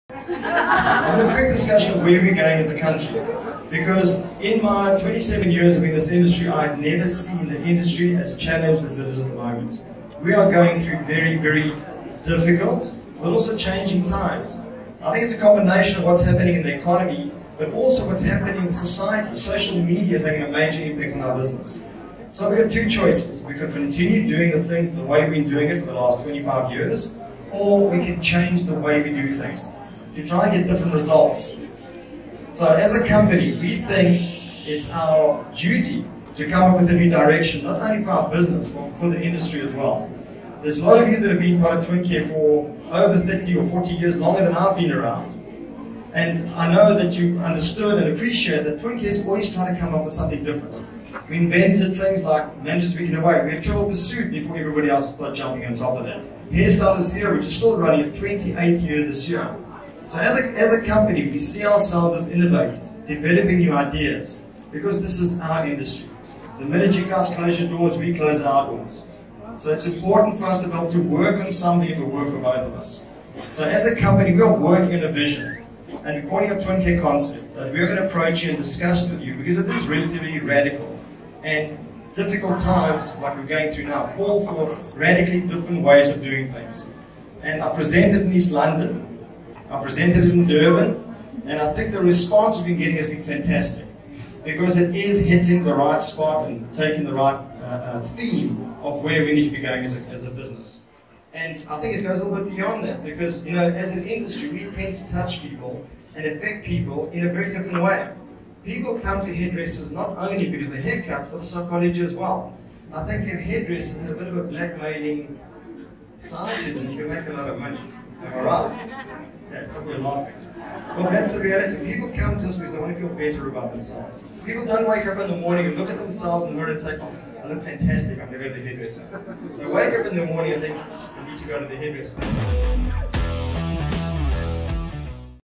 Watch: Stav Dimitriadis Speaks at Paul Mitchell’s The Event
Industry icon and Twincare CEO Stav Dimitriadis has his finger on the industry’s pulse. Here’s his take on where our industry is at the present time. An inspirational and thought provoking watch.